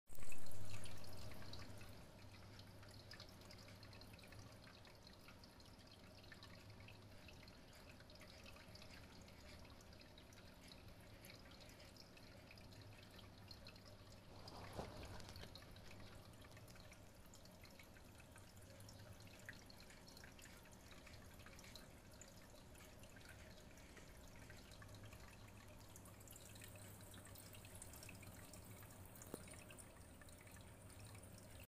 Feeding orna jr..
Earth worm, meal worm he's fav. 30cm tank, filtered, black larva rock, temp 28°. He's doing great..